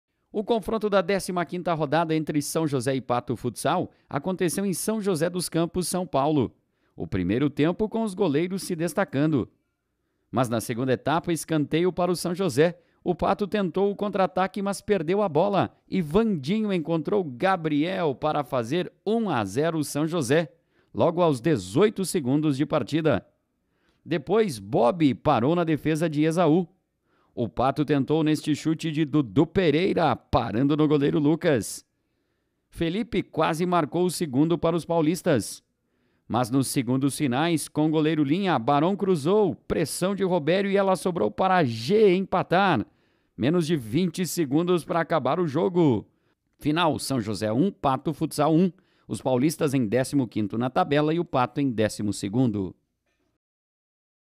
Jogando em São Paulo, o Pato Futsal busca empate nos segundos finais
Esportes: Final de jogo, São José 1 e Pato Futsal 1. Os paulistas em décimo quinto na tabela e o Pato em décimo segundo.